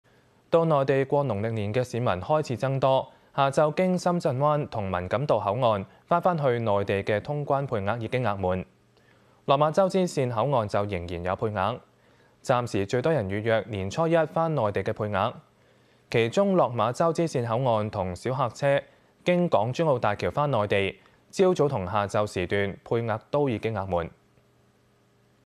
香港新聞｜無綫新聞｜20/01/23 要聞｜下午經深圳灣及文錦渡往內地配額已滿 落馬洲支綫口岸仍有配額｜ TVB News
到內地過農曆年的市民開始增多，下午經深圳灣及文錦渡口岸回內地的通關配額已額滿，落馬洲支綫口岸仍有配額。

暫時最多人預約年初一回內地的配額，其中落馬洲支綫口岸，及小客車經港珠澳大橋回內地，早上及下午時段配額都已額滿。